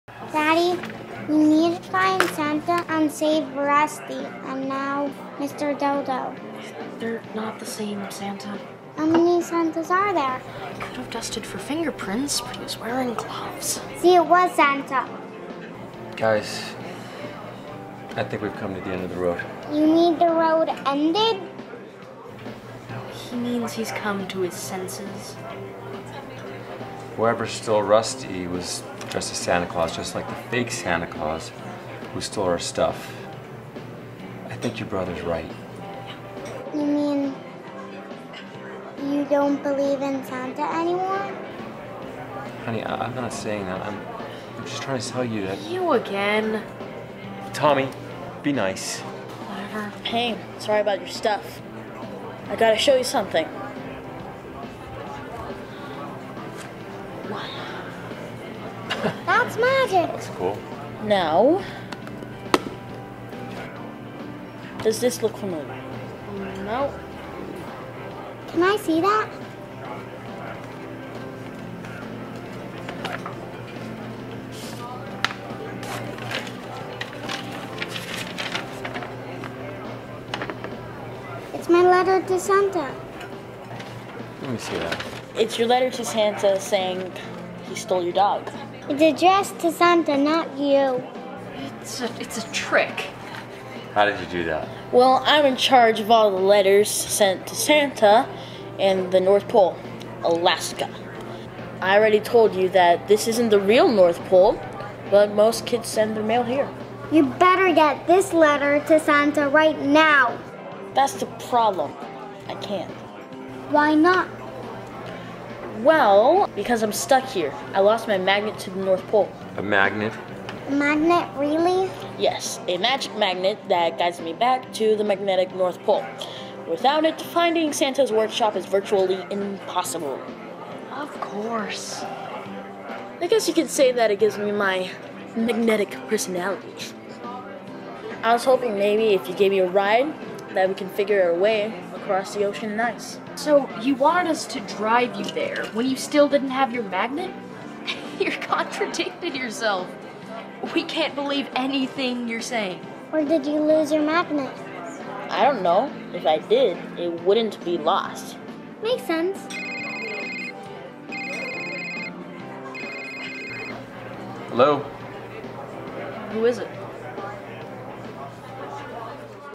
Scene 80 Diner rough Cut new QT
New rough cut Scene 80 in Diner
Starring Yves Bright, Chase Pollock, Lexi Golden & Samuel Portugal